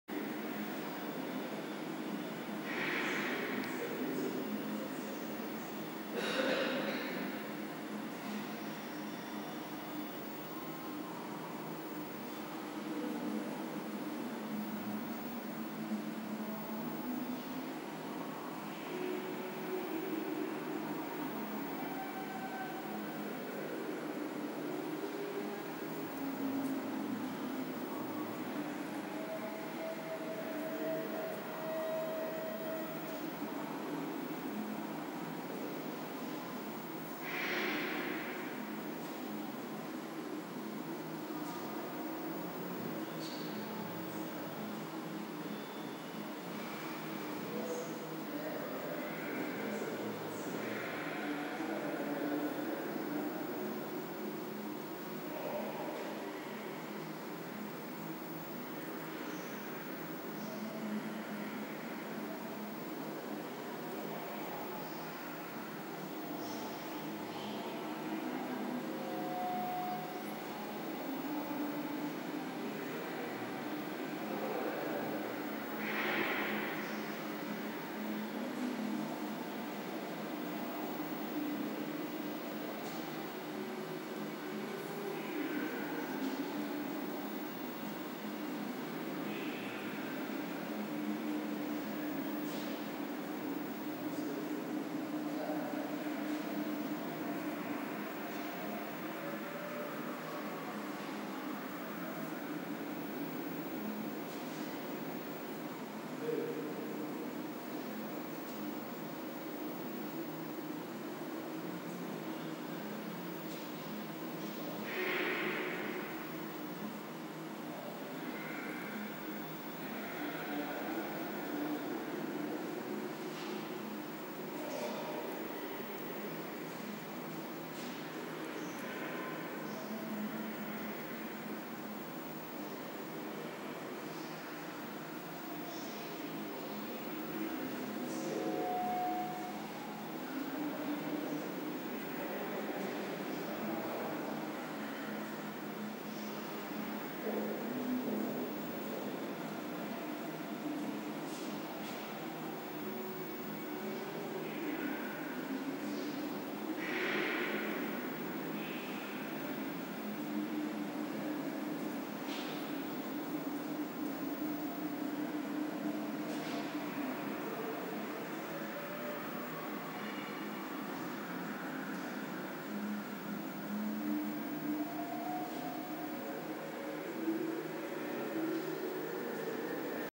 Andreas Korte - Installation Akademiegalerie
Ausschnitt der Videoinstallation in der Akademiegalerie Düsseldorf.
Die Erfindung der Wirklichkeit - Photographie an der Kunstakademie Düsseldorf von 1970 bis heute. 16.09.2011 bis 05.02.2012